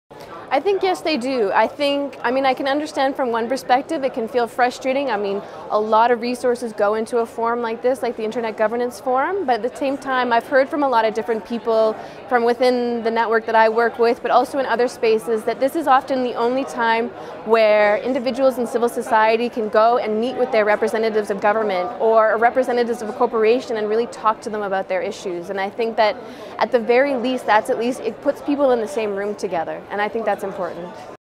Global IGF 2017: Shawna Finnegan on Multistakeholder Model
Shawna Finnegan, logistics coordinator for Association for Progressive Communications, shares their answer to the question, “Do multistakeholder efforts like IGF have any impact on the processes they are created to positively influence?” Recorded at the Global Internet Governance Forum 2017 in Geneva, Switzerland, by the Imagining the Internet Center, an initiative of Elon University, North Carolina, USA.